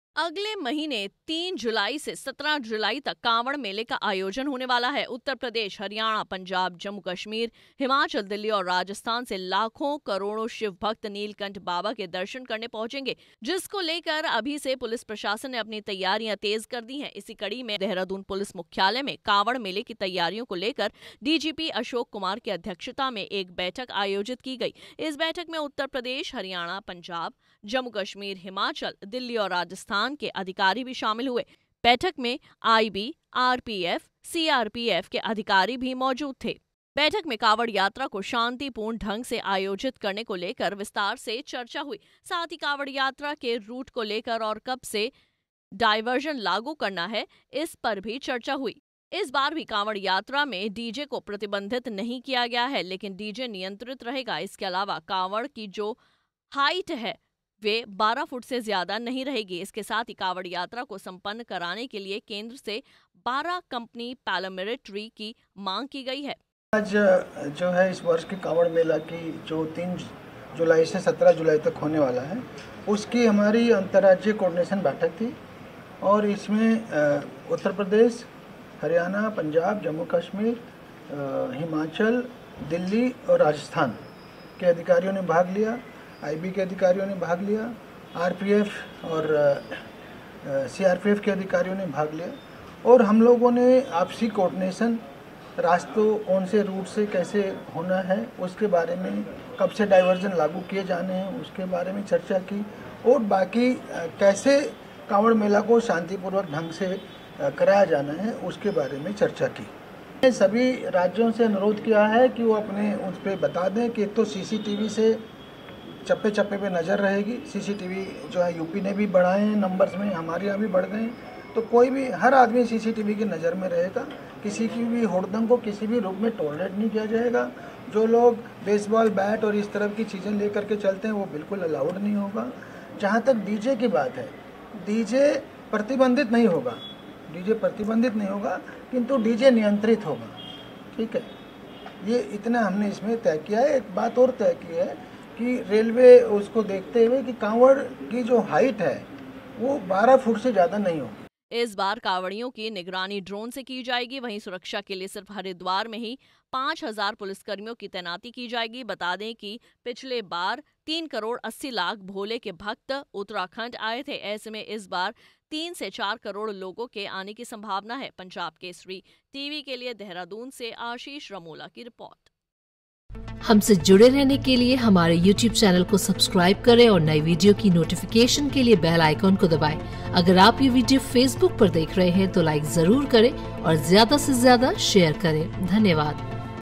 Kanwar Yatra 2023: इस बार भी कावड़ यात्रा में डीजे पर रोक नहीं, ड्रोन से होगी कांवड़ियों की निगरानी
अगल महीने तीन जुलाई से 17 जुलाई तक कांवड़ मेले का आयोजन होने वाला है. उत्तर प्रदेश, हरियाणा, पंजाब, जम्मू कश्मीर, हिमाचल, दिल्ली और राजस्थान से लाखों करोड़ों शिव भक्त नीलकंठ बाबा के दर्शन करने पहुंचेंगे...जिसको अभी से पुलिस प्रशासन ने अपनी तैयारियां तेज कर दी है..